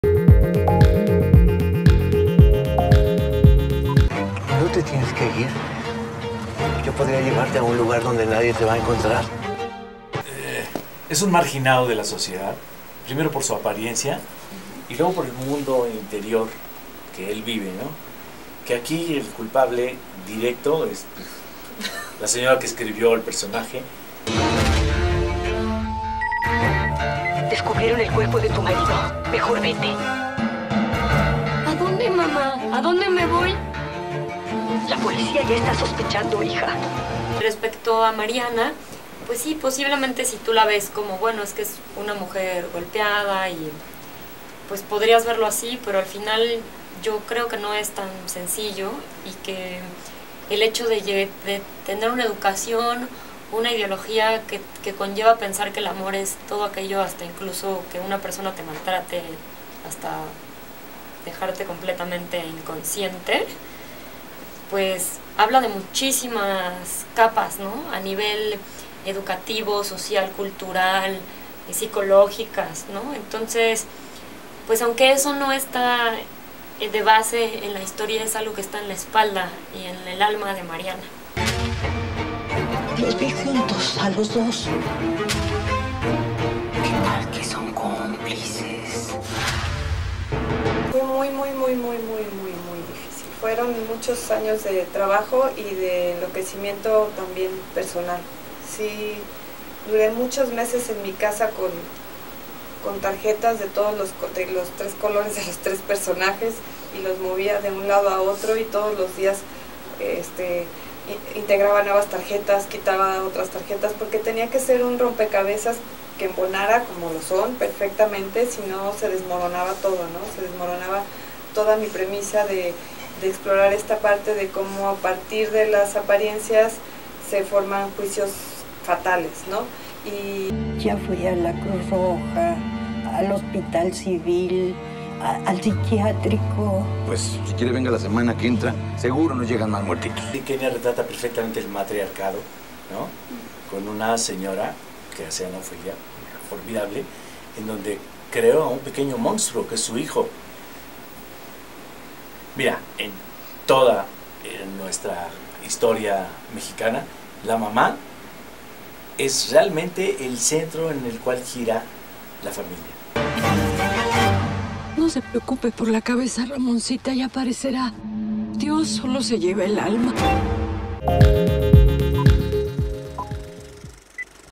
Fecha de Caducidad - Entrevista con los actores y la directora
Damián Alcázar, Marisol Centeno y Kenya Márquez nos hablan de la película Fecha de Caducidad.
No se la pierdan!